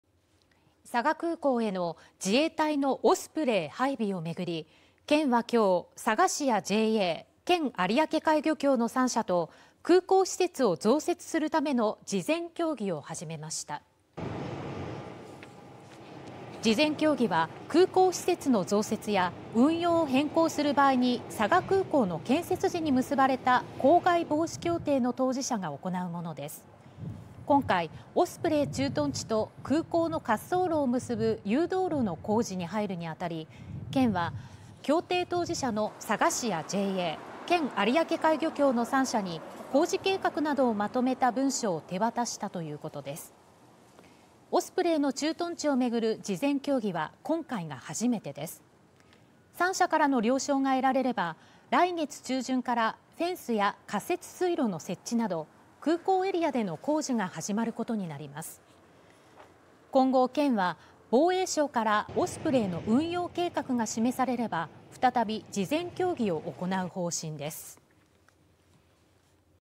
オスプレイ配備 佐賀市・JA・県有明海漁協の3者と空港施設を増設するための事前協議 初開催【佐賀県】 (24/01/29 18:40)
佐賀空港への自衛隊のオスプレイ配備をめぐり、県は29日、佐賀市やJA、県有明海漁協の3者と空港施設を増設するための事前協議を始めました。

事前協議は、空港施設の増設や運用を変更する場合に佐賀空港の建設時に結ばれた公害防止協定の当事者が行うものです。
今回、オスプレイ駐屯地と空港の滑走路を結ぶ誘導路の工事に入るにあたり、県は協定当事者の佐賀市やJA、県有明海漁協の3者に工事計画などをまとめた文書を手渡したということです。

オスプレイの駐屯地をめぐる事前協議は今回が初めてです。
3者からの了承が得られれば、2月中旬からフェンスや仮設水路の設置など、空港エリアでの工事が始まることになります。
今後、県は防衛省からオスプレイの運用計画が示されれば、再び事前協議を行う方針です。